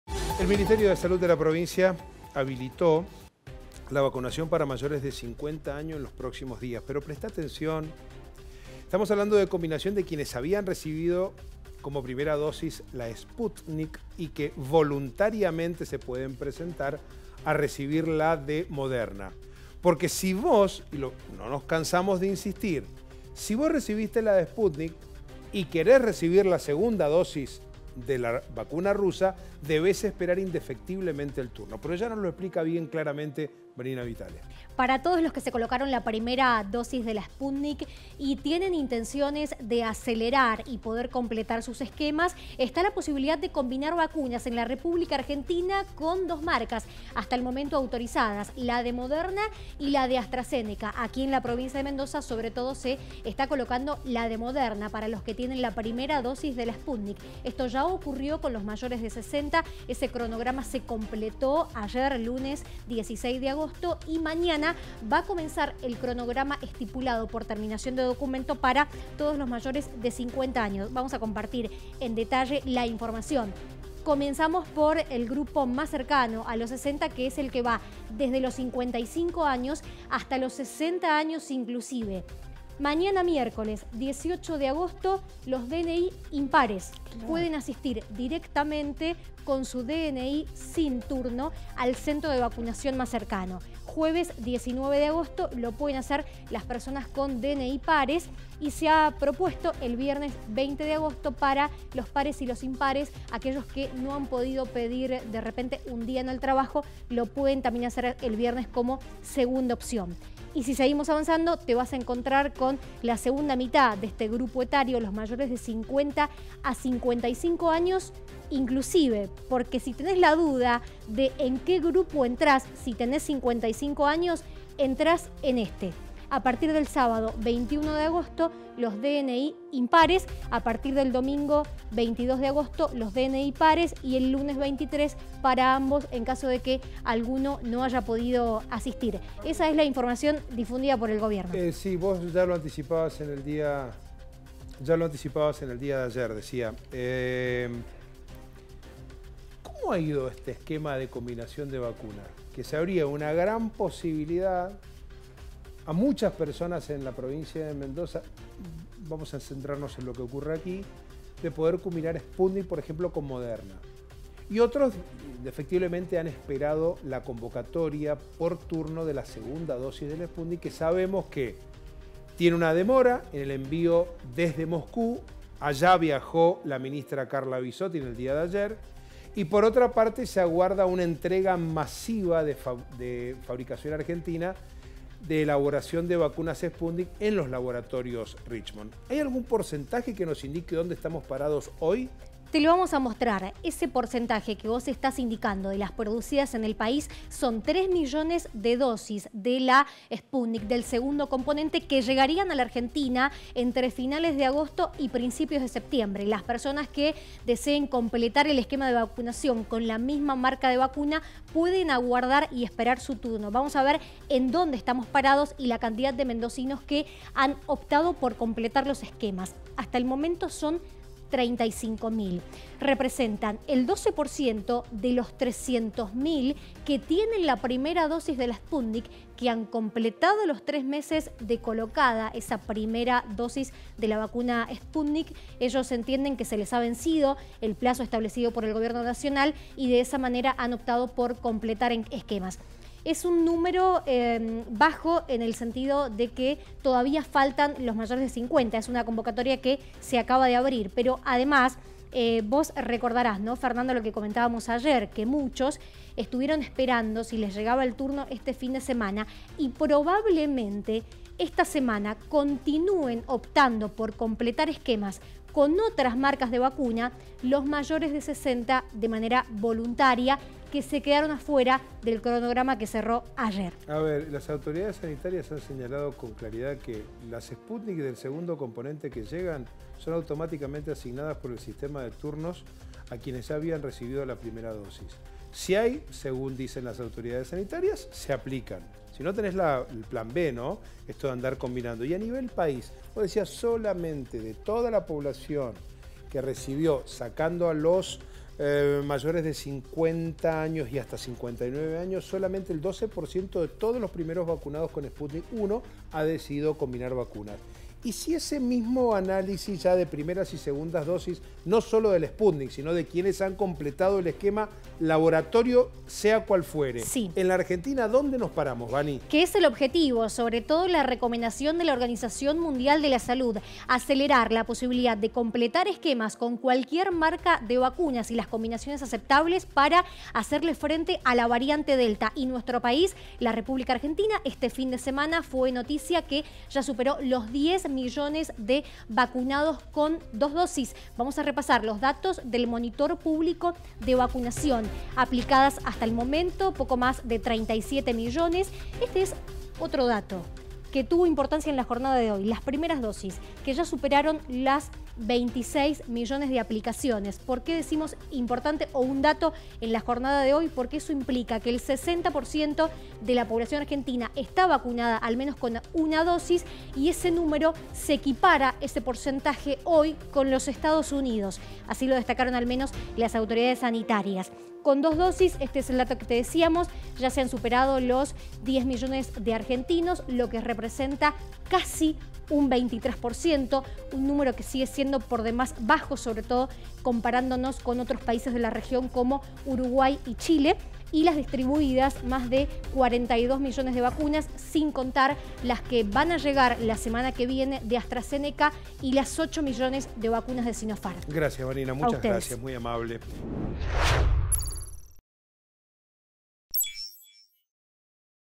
Vacunas combinadas: cómo es el cronograma de mayores de 50
Habilitaron la vacunación combinada para mayores de 50 años. Es para las personas que recibieron la primera dosis de la Sputnik V y que ya cumplieron tres meses. Se convocará a partir de mañana según la terminación del DNI.